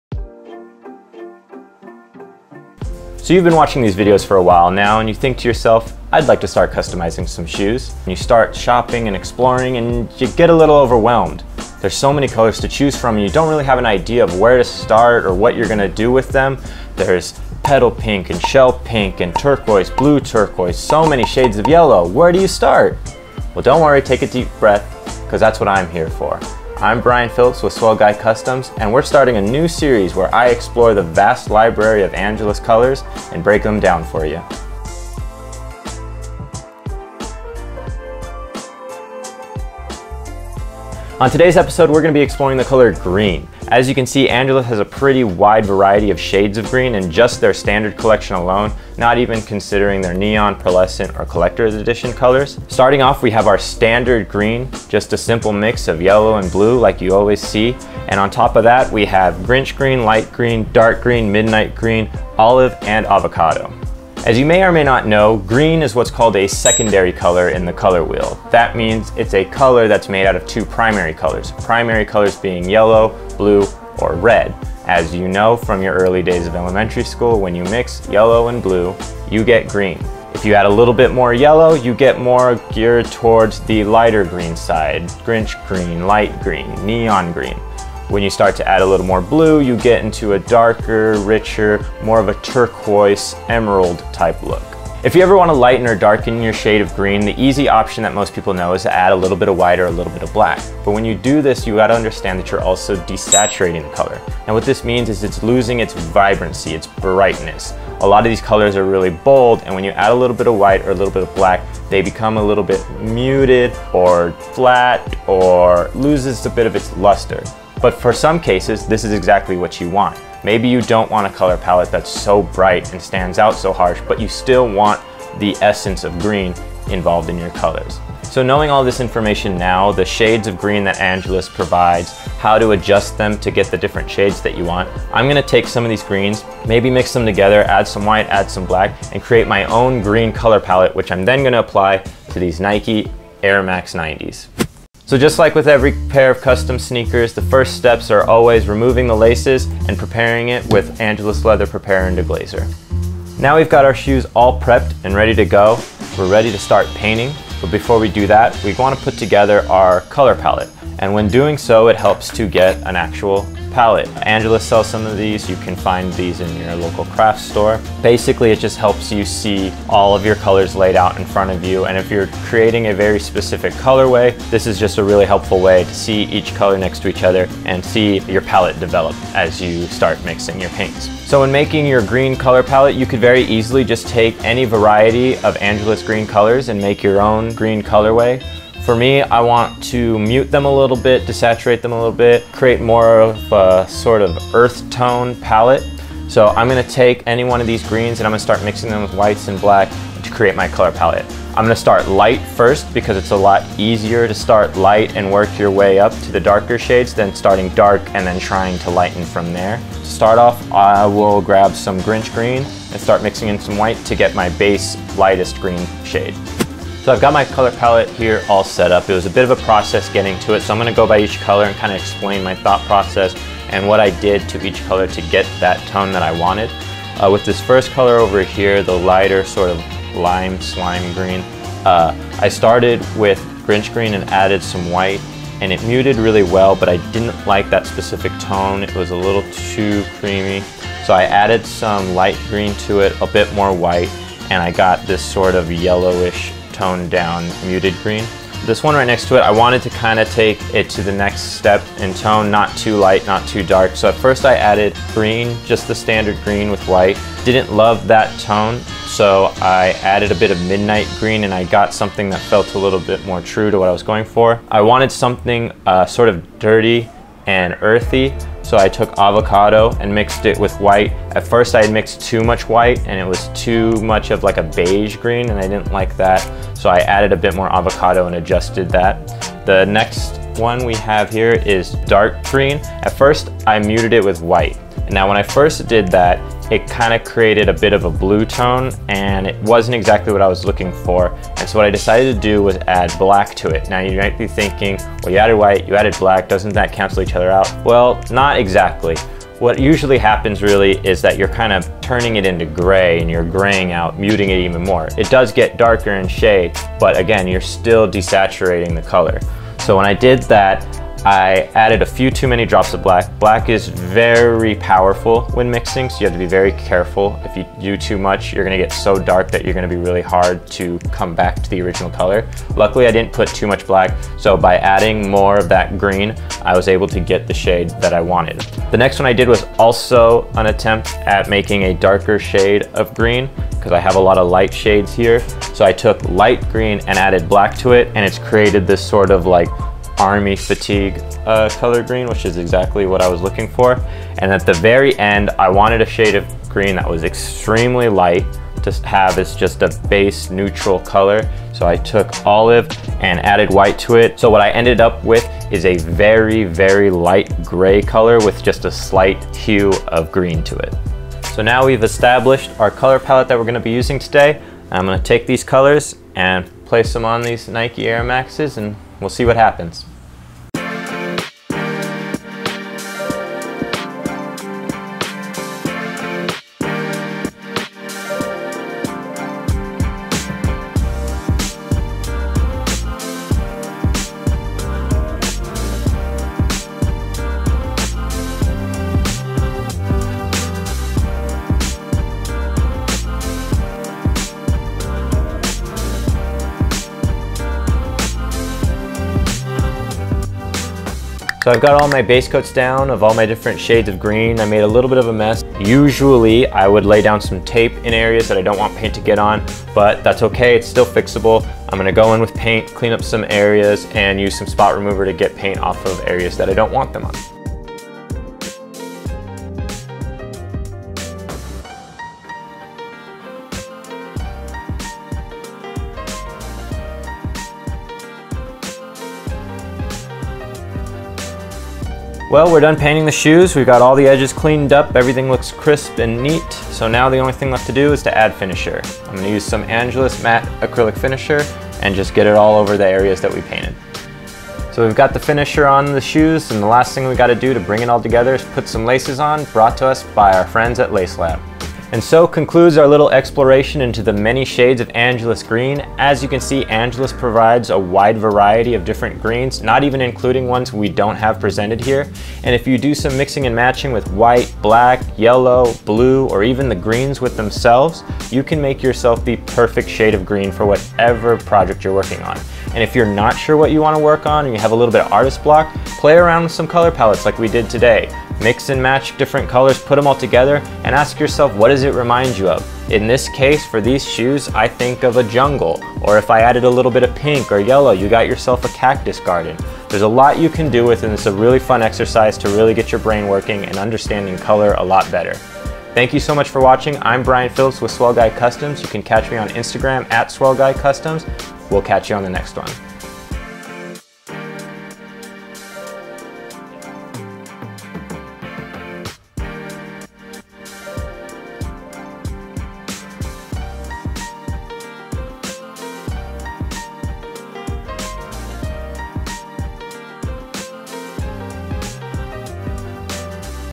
Custom Green Tones Shoes | How to Mix Angelus Paints
Bryan (@swellguycustoms) is back again, and this time he is teaching you all about greens, and how to mix them for your very own custom palette. Bryan walks you through mixing a variety of Angelus greens to create custom tones, and he shows you how the possibilities are truly endless. So grab some paint, hit play, and follow along as you get the 101 on creating your very custom shoes with 100% custom mixed colors.

0:00 - Intro
0:34​​ - B-Roll
0:40 - Angelus Standard Greens
1:04 - All About Green
1:35  - Lightening and Darkening Green
2:24 - Prepping the shoe with Angelus Preparer and Deglazer
2:33 - A Rundown of What’s to Come
3:36 - How Bryan Achieved His Color Palette
6:01 - Painting the Shoes
6:45 - Cleaning Up the Shoe with Spot Remover
7:12 - Adding an Acrylic Finisher and Lace Lab Laces
7:21 - Outro
8:23 - B-Roll